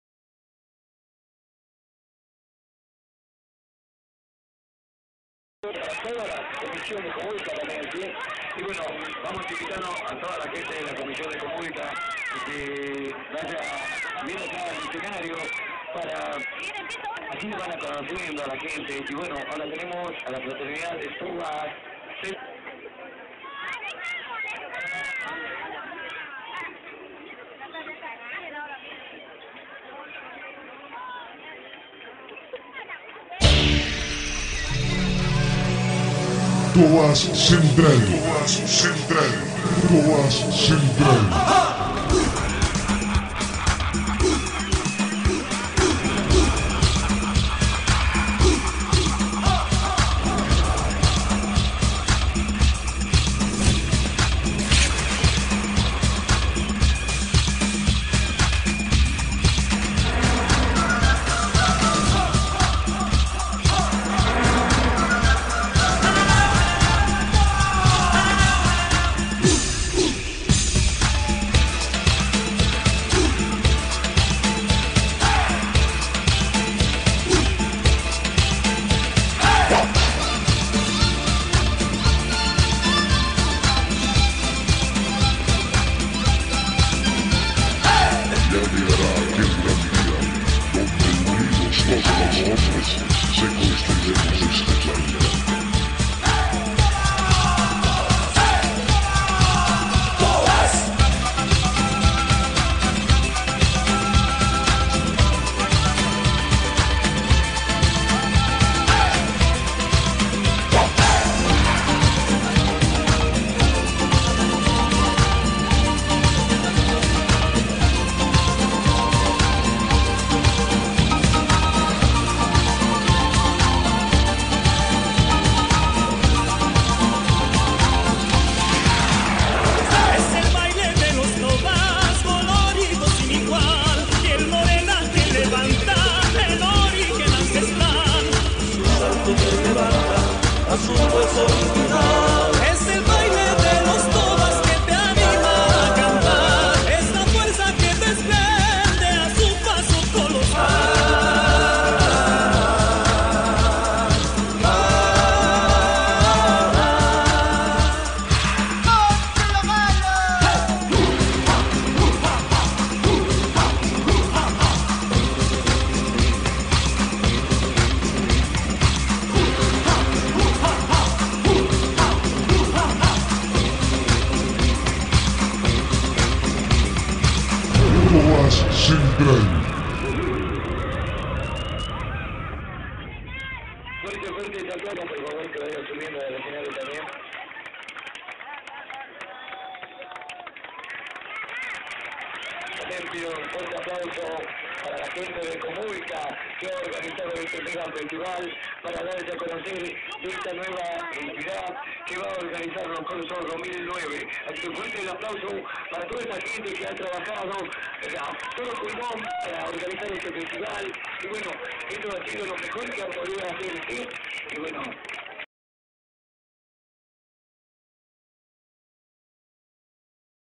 "FRATERNIDAD TOBAS CENTRAL SALTA" Anfiteatro Parque Gral. San Martin (27-04-08)
Video nuestra actuación en el ANFITEATRO del Parque General San Martin, en la muestra de Agrupaciones del carnaval que realizo la Asociacion CO.MU.yCA.(Comparsas Murgas y Caporales) el Dia 27 de Abril del 2008.
Agradecimientos a los bailarines: NAHIR- TULMY- MERY- CRISTIAN y ARIEL, .
Fraternidad Tobas Central Salta
Posteado By CHIMANE